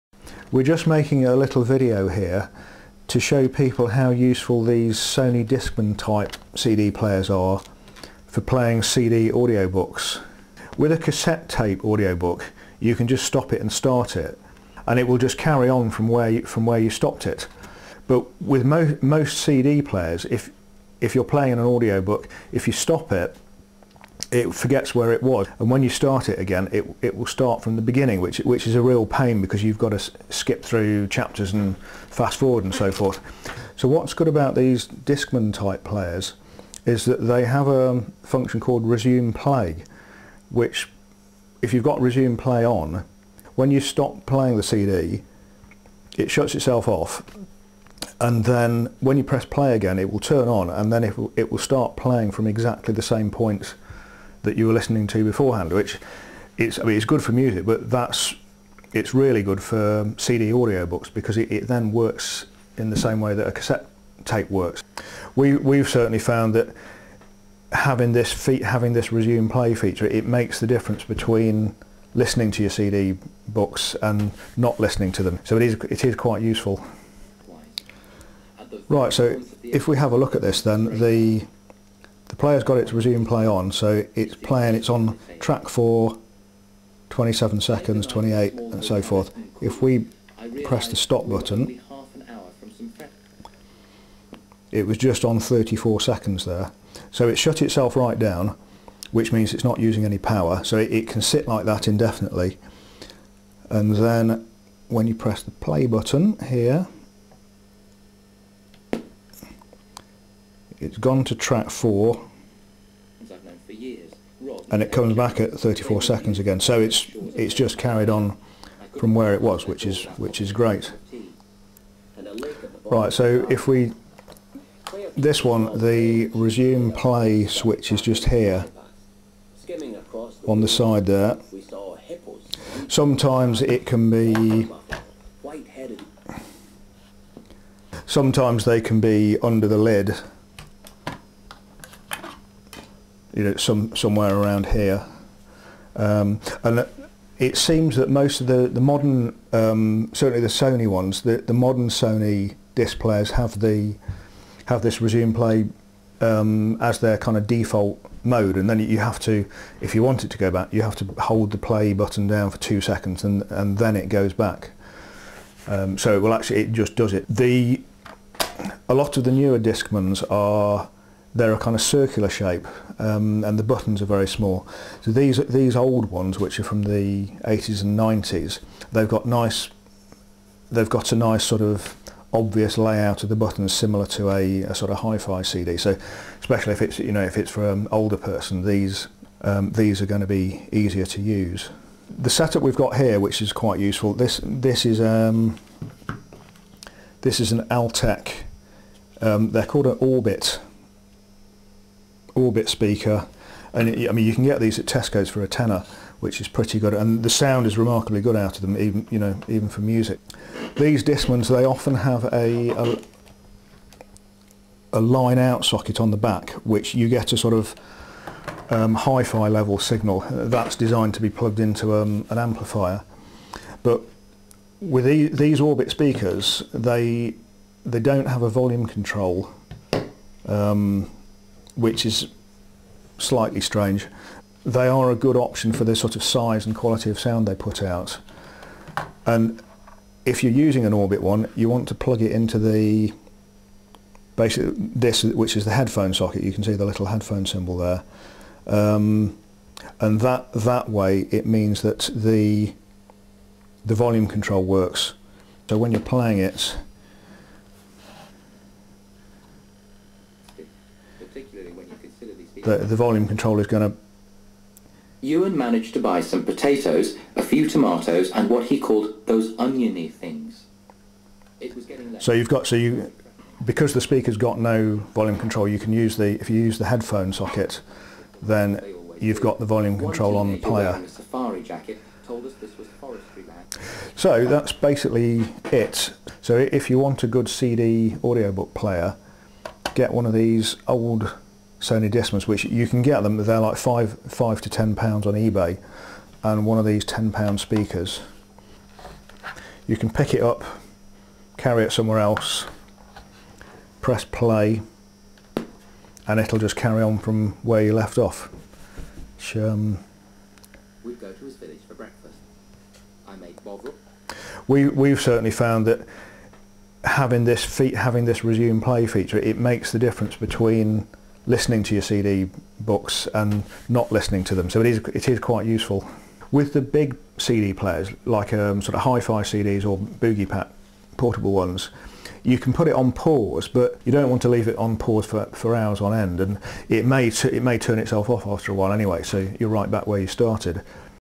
Sony Discman CD Audio Book Player 'Resume Play' re-starts CD from where you stopped, like a tape
The Resume Play feature you get on  many (not all)  Sony Discmans (ie CD Walkmans)  is very useful for playing CD audiobooks. With 'Resume Play', when playing a CD, when you press stop, the discman stops and turns itself off.  When you press play again, it starts the CD from the point at which you stopped, the same as a cassette tape would play, and not from the beginning. Very handy

For some reason, boogie pack and Hifi type CD players do not normally have resume play -- you can put them on pause, but  it is not ideal to do this for long periods

To see if a Discman has the resume feature, you can find and check the user manual for many Discmans at this url:

Select Product category -- 'CD Walkman' then the series and model number
Don't have resume:
D-130, 131, 132CK, 133 -- early model -- great buttons
Series D-E220 / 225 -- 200′s don't resume

Have a Resume slide-switch:
Traditional layout with flipup lid and CD player buttons -- car kit version has a lighter socket 12v to 4.5v adapter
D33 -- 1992
D-E300, 301, 305 , 307CK   (have nice layout)
Sony DE 400 / 401 / 403, 405 / 406  409, 446CK  (CK = car kit)
D-E405  is a good choice -- 1998 has resume + lineout

Models with Resume play as default:
More recent and current Discmans have Resume Play as default -
When you press play after having stopped,  resume play is the default and you have to hold the play button down for 2 seconds to get it to start from the beginning
D-E330, 340, 350, 351, 356ck
Circle shaped -- D-EJnnn series eg EJ915 and D-FJnnn series have it built in
We assume they all have, but you can check on the sony website above

Other notes:
The 90′s  Discmans won't play CD-R's that you have made yourself  -- later models will play CD-RW's. This is useful for playing copies of CD's
D-EJ915 has  internal proprietary flat rechargeable NiMh batteries

Other Makes -
There are lots of other makes of discmans -- we haven't checked for resume play
Technics SL-XP505 (nice player) says it has resume, but it resumes from the start of the track / chapter you were on, which is not nearly as good, as you have to fast forward to the point you were at